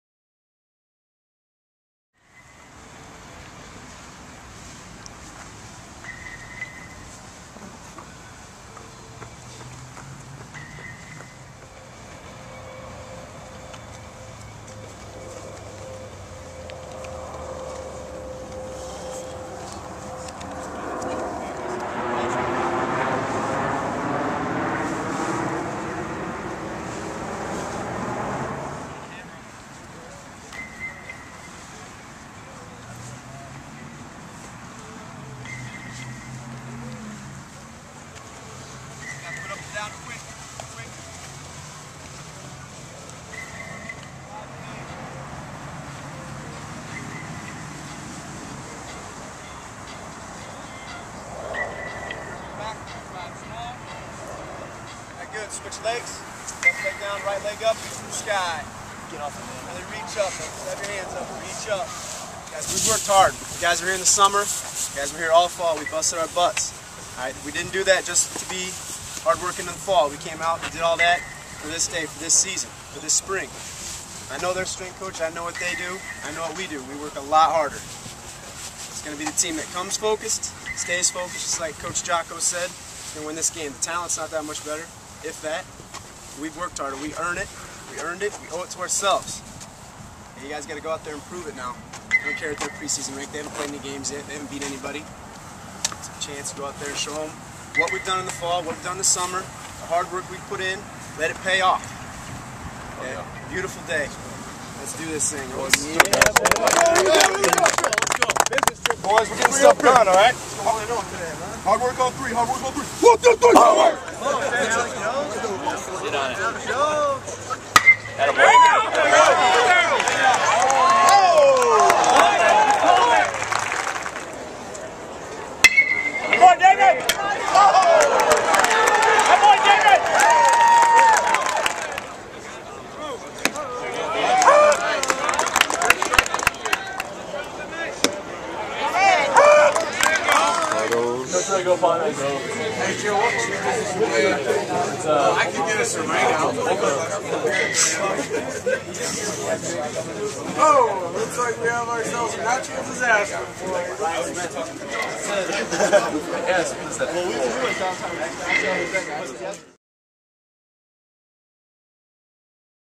2010 NIU Huskies Baseball at Arizona State - Trip Highlights
Northern Illinois baseball traveled to Arizona State to face the fifth-ranked Sun Devils.